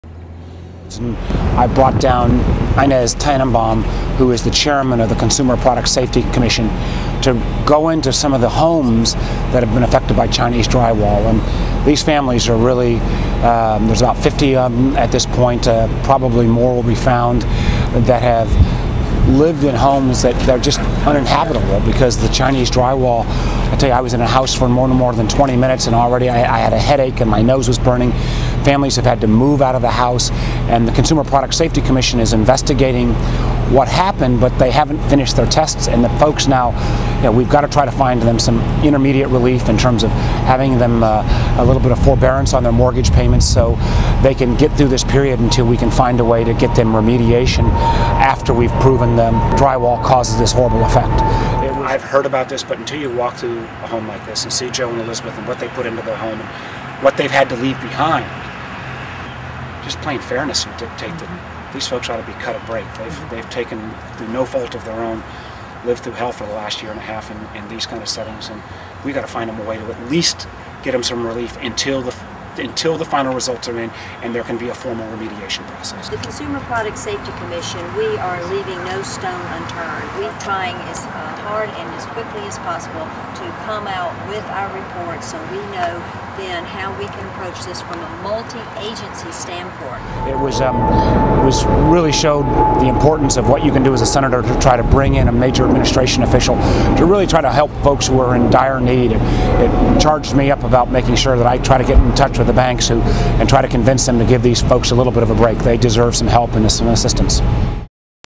Mark Warner visits Chinese drywall victims
Senator Mark Warner traveled to Hampton Roads to meet with families who own homes damaged by toxic Chinese drywall, and he brought the chairwoman of the U.S. Consumer Product Safety Commission, Inez Tenenbaum, with him.